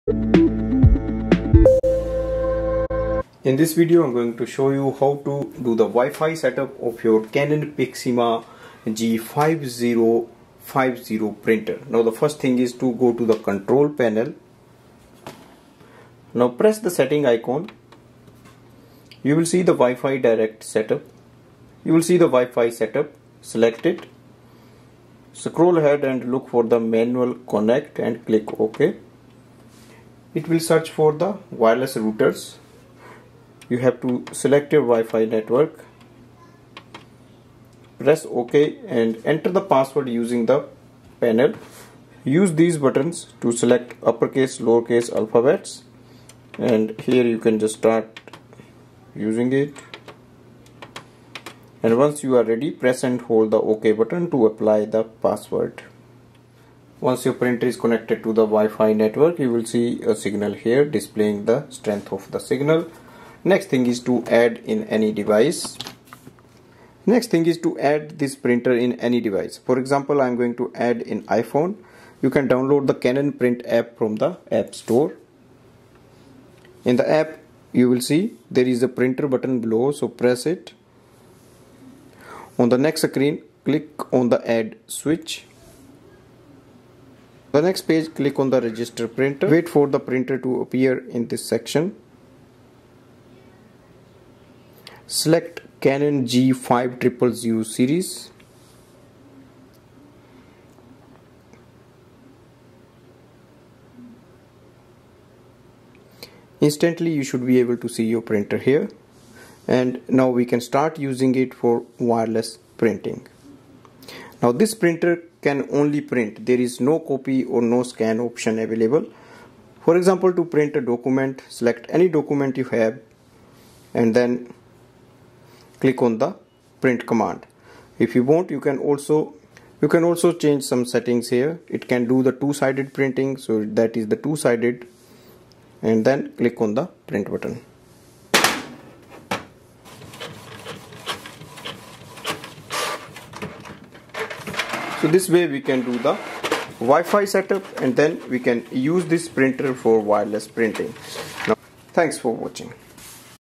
Canon Pixma G5050 WiFi Setup
This video reviews the method for the wireless Wifi setup of your Canon Pixma G5050 Printer. This helps to connect the printer to the home or office wireless network using wifi setup and add in the iPhone for wireless printing.